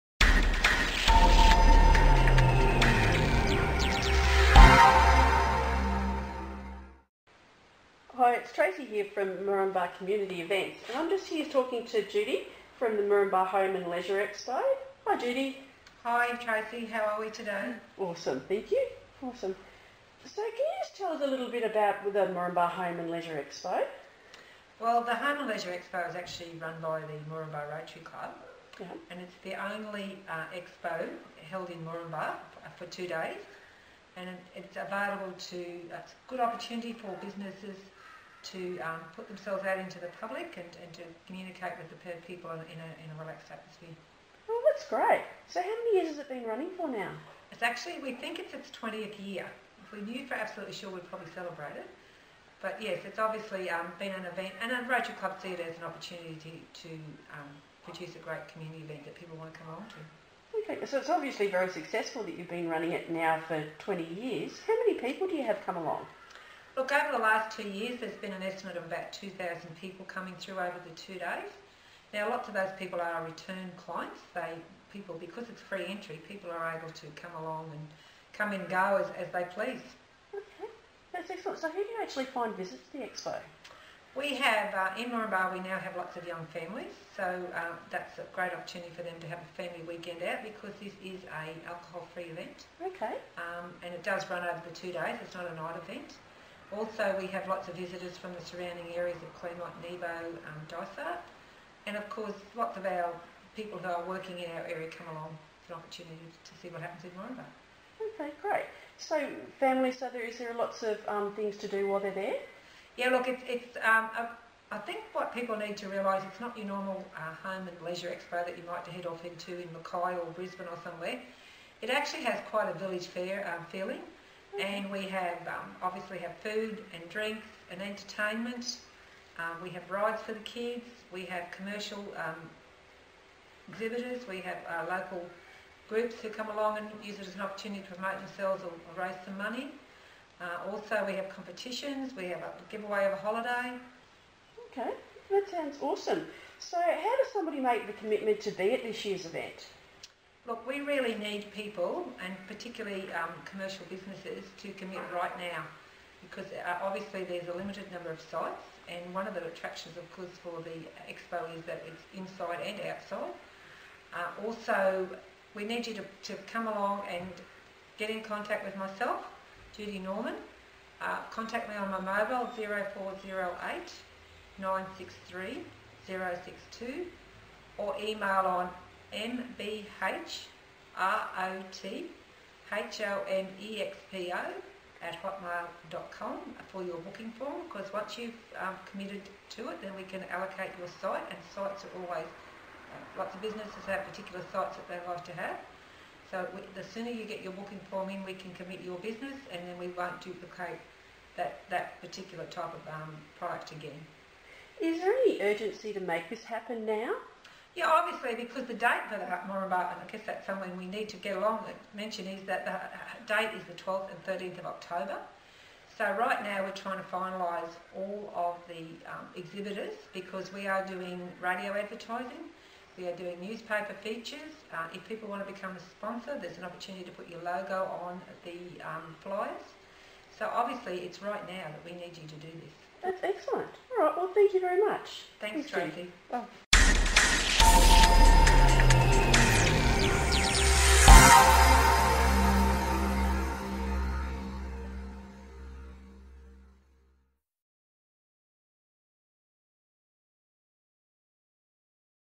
Moranbah Home Leisure Expo | Rotary Club of Moranbah | Moranbah Event | Judy Naumann shares
Moranbah's local Rotary Club brought you the Annual Moranbah Home Leisure Expo for 2013.
Judy Naumann shares what has made this a great local Moranbah Community event for the past 20 years, and is still going strong!
The years event that was held on the 13th & 14th of October 2013 at the Moranbah Community Center was another great success for Moranbah.
People travel from throughout the Isaac region to see Exhibitors showcase their best Products and Services to an estimated 2,000 visitors.
With many great exhibits on display again for 2013 from the local Moranbah area and throughout the state - Commercial & Home Based Businesses, Many local Moranbah Community based Groups like Relay for Life, Moranbah Scouts were there to provide information about what you is happening with these groups in town and have some fun and fundraising over the weekend .
Lots of great entertainment, food & rides.
Great offers for visitors to the Moranbah Event. Plus many samples and free advice. Also prizes and giveaways during the weekend.There is a holiday as the major prize!
Thanks to the Event Sponsors - Isaac Regional Council, Moranbah Community Workers Club, CMFEU, Isaac Plains Coal, HCE Electrical and Long Island Resort.
Sites are limited to ensure that exhibitors are given an exclusive opportunity at the event - this makes this a unique opportunity for Businesses to get themselves out in the community and provides the visitors more variety in what's on show for them to see and experience.
There is also great advice about Moranbah real estate and property development and investment advice if you are looking to secure your families future.
Some regional schools are on hand to share what the have to offer without having to travel into Mackay.
Any exhibitors that have not yet registered are encouraged to get in now so they don't miss out for the next event.

Exhibitors for Moranbah Home Show 2013 is Graham Bowling, Top End Tanks, Rack N Stack, Freshly Squeezed, High Country Enterprises, Vitagroup(Telstra), Lindsay Waugh, Century 21, Steve Taylor, Cathedral College, Multi Rack, World Creations, Potato Max, Whitsunday Sales, Donna Williams, Norwex, Moranbah Craft Club, Moranbah Scouts,
Anglo American, Great Barrier Reef Marine Park,Coco and Lily, Jenny Kinderis,Queensland Academy of Maths and English, Moranbah QCWA, Arrow Energy,
Blood Sugar and Blood Pressure Testing and lots lots more.